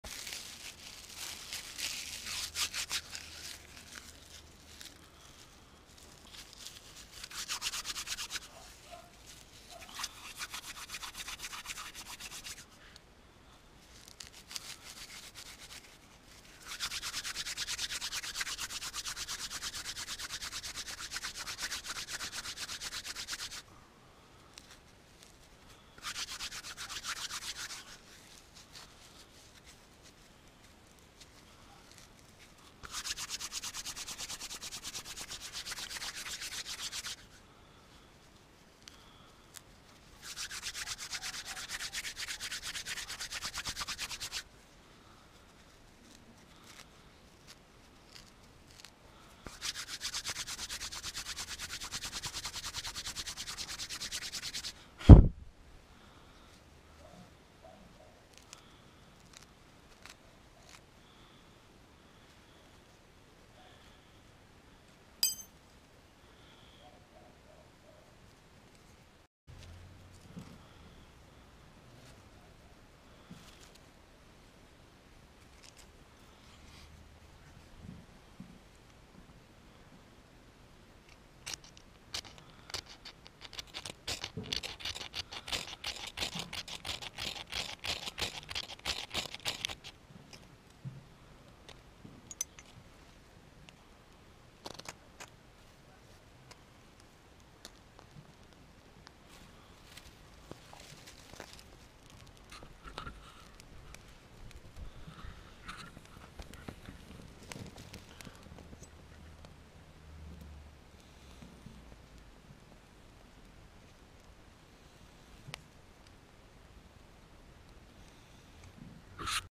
Cremene, amnar, flint pentru aprins focul, supravetuire, drumetii, aer liber.
Comenzi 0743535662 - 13 lei buc. comanda minima 2 bucati.

Specification:

-Material: Flint , magneziu, scraper.

-Diametru bara: 5 mm

-lungime totala: 65mm

-Lungime bara: 38mm

-Greutate: 16g

Această unealtă este foarte utilă în situaţii de supravieţuire, atunci când focul este esenţial pentru căldură, semnalizare, alungarea animalelor şi prepararea hranei. Este utilă la pescuit, la vânătoare, camping, drumetii montane, supravietuire in orice clima.

Amnarul îţi dă siguranţa că oriunde te-ai afla, chiar dacă te prinde ploaia, ai ceva cu care să aprinzi focul.

Funcţionează în condiţii de umezeală şi temperaturi scăzute fiind foarte uşor de folosit.

Produce scântei(magneziu topit) la temperaturi mari care aprind iasca, iarba uscată, hârtie, etc. (depinde de imaginaţia celui care aprinde focul).

Comanda minima 2 bucati la amnarele mici

La 4 amnare mici cumparate unul graris.

Pret 13 lei buc.

Trimitem in toata tara parin posta sau curier.